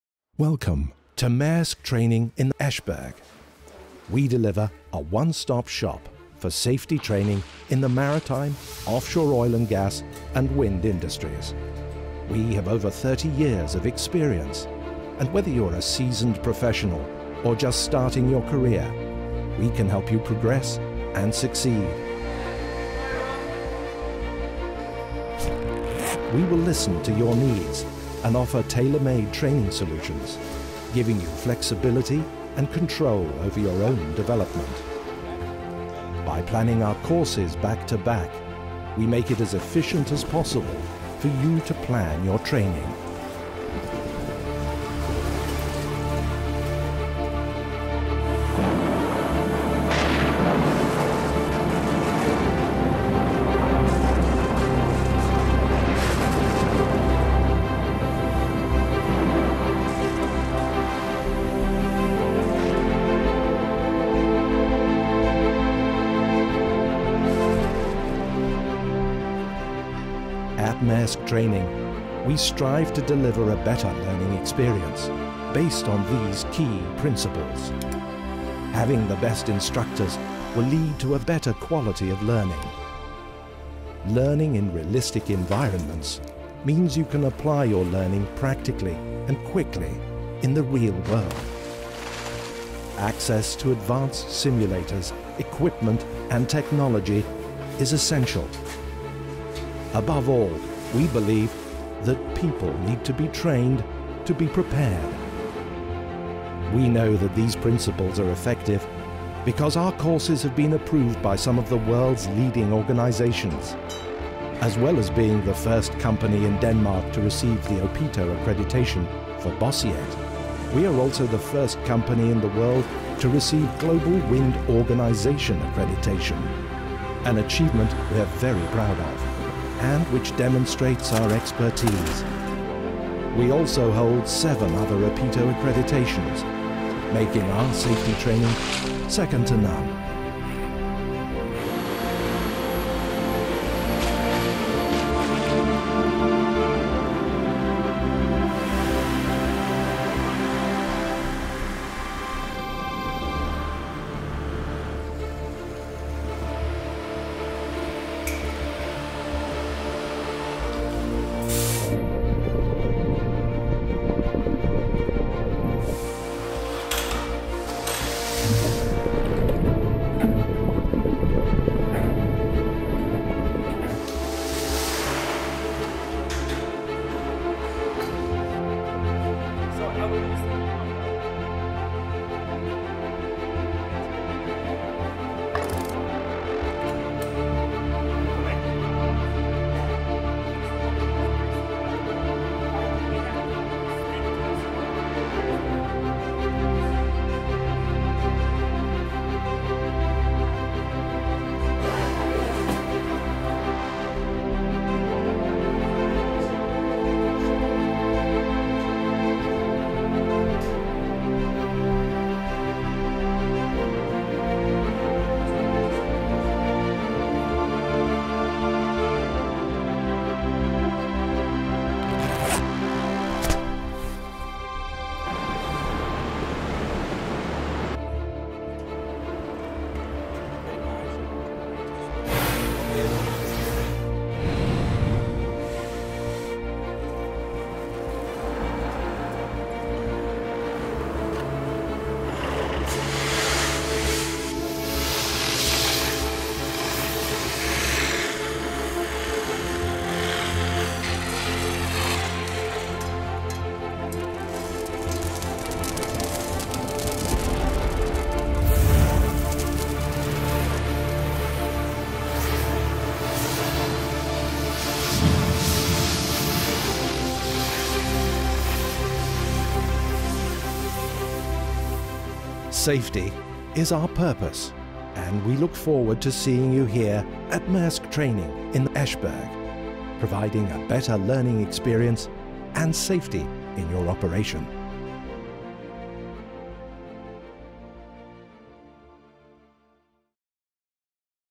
Offshore safety and survival training in Esbjerg, Denmark
Service, flexibility and quality are the key words at Maersk Training in Esbjerg. Our training focus lies on the oil & gas, maritime and wind industries delivering learning that ensures safety of the operational employees in the industries. We also train specific technical knowhow for the wind industry that is fundamental for performing work at the required level of competence.